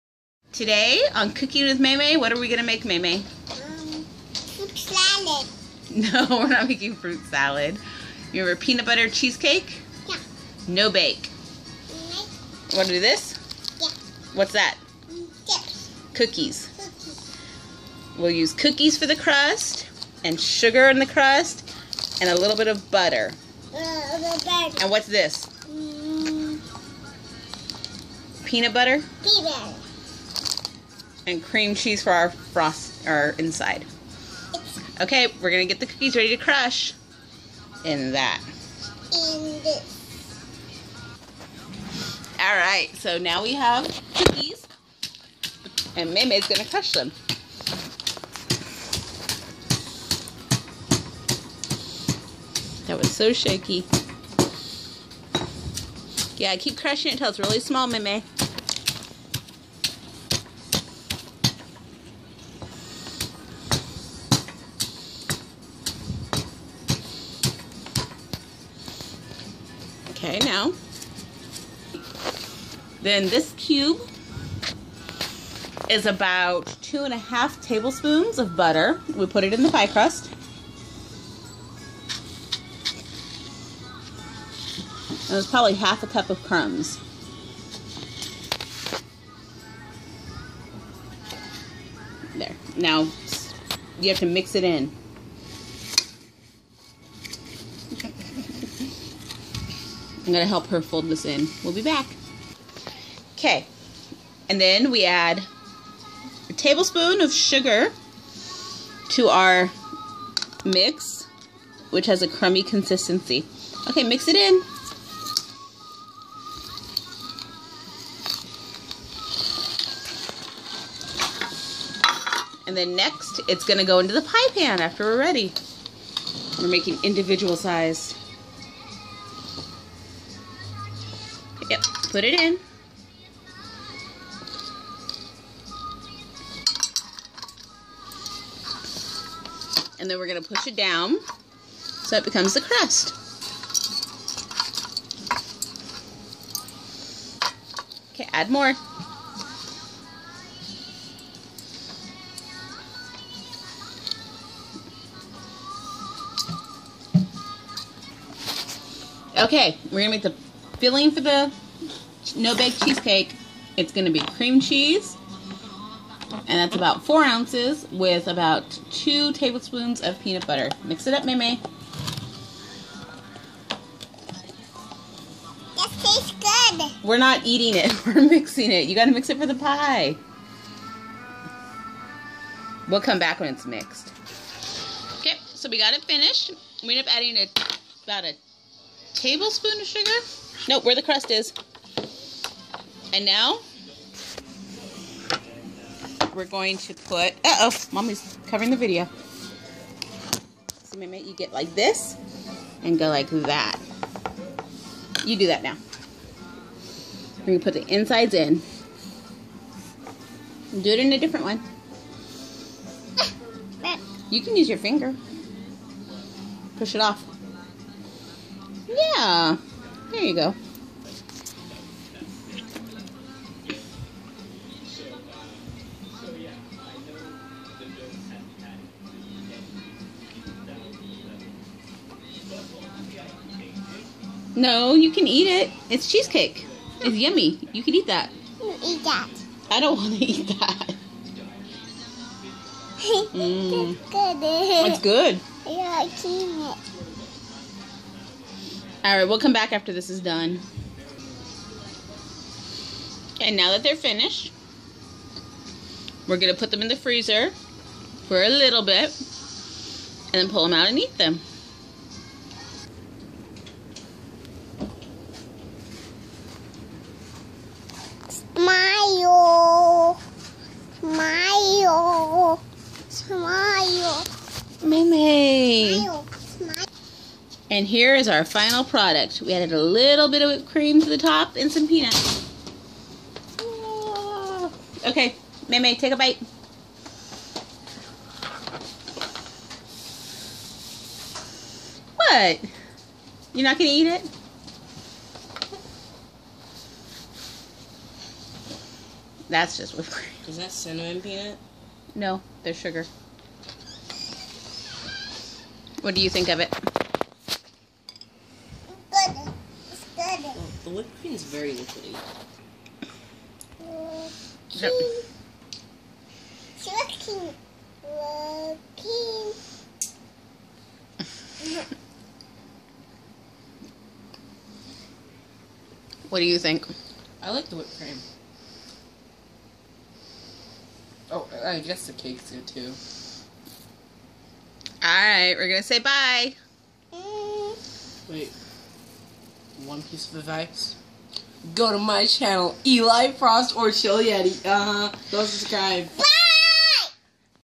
Cooking with Mai Mai: No-bake Peanut Butter Cheesecake
A quick home made recipe for this. It probrably could of used some chocolate chips but we didn't have any.